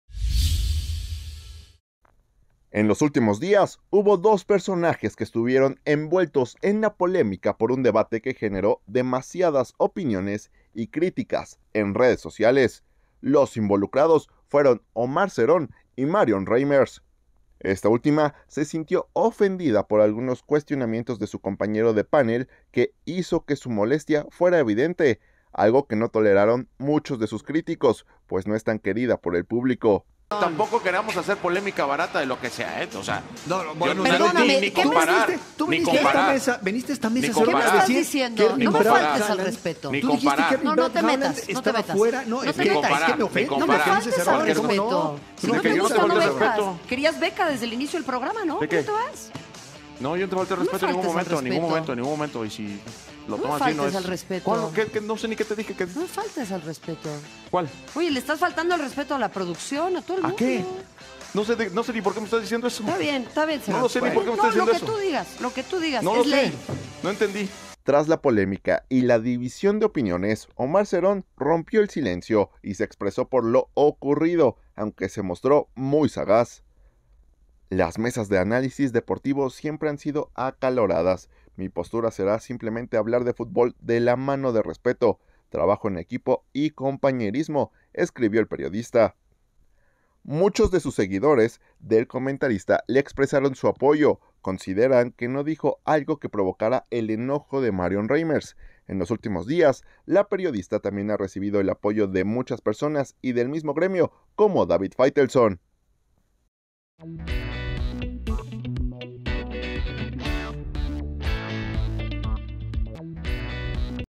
Omar Zerón da su versión tras polémica con la conductora Marion Reimers
Ambos comentaristas protagonizaron un momento polémico en una mesa de análisis de la Champions League